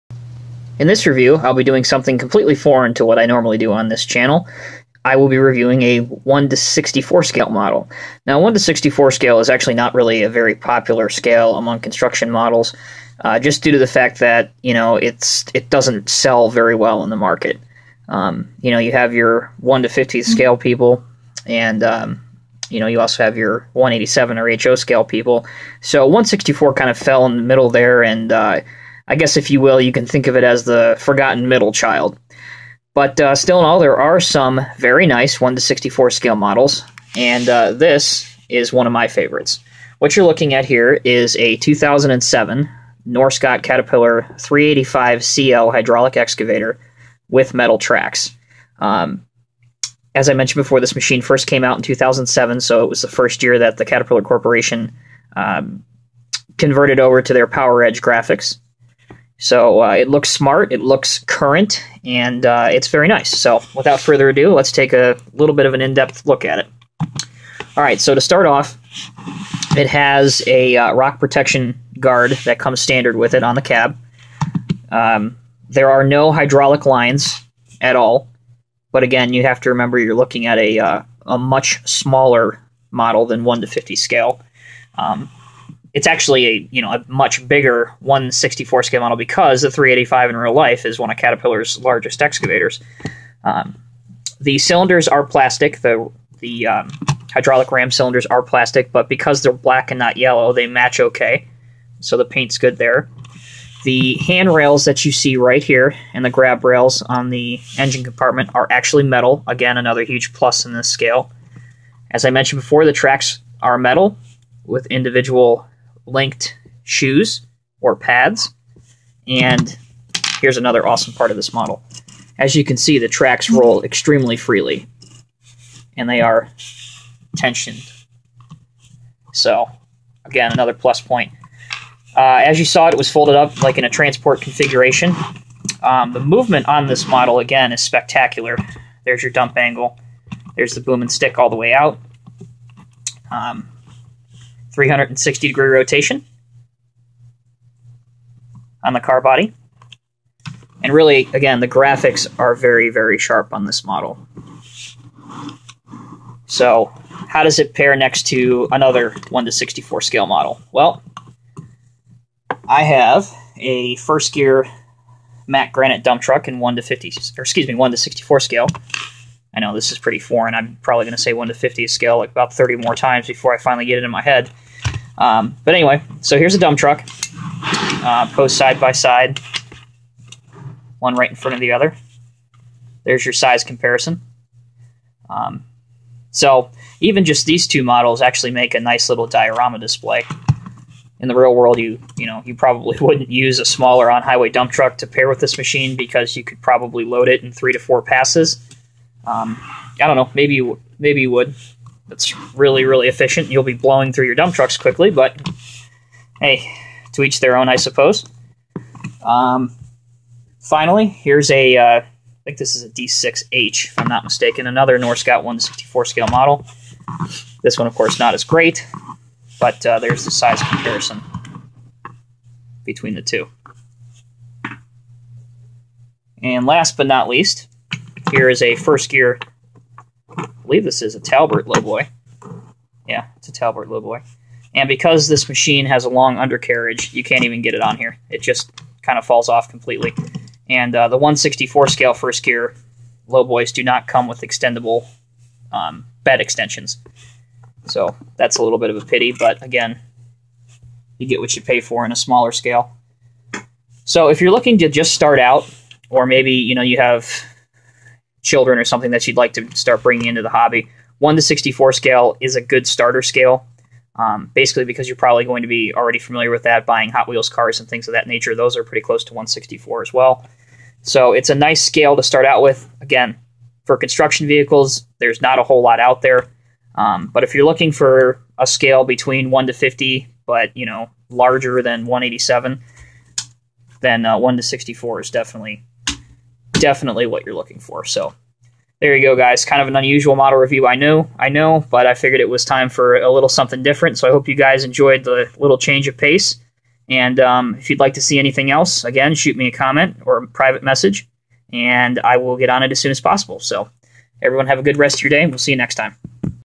Norscot Cat 385C L Excavator 1:64 Scale
A refreshing break from normality this review looks at a 1:64 scale excavator made by Norscot.